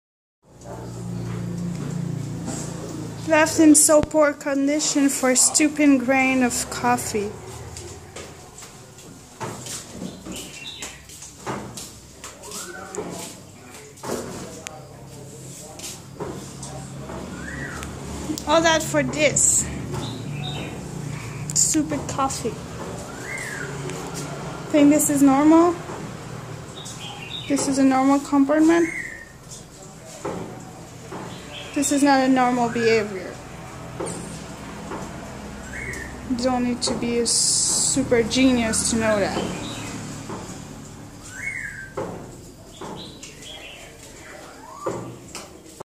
Don't buy weasel coffee
A farm of weasel coffee in Vietnam.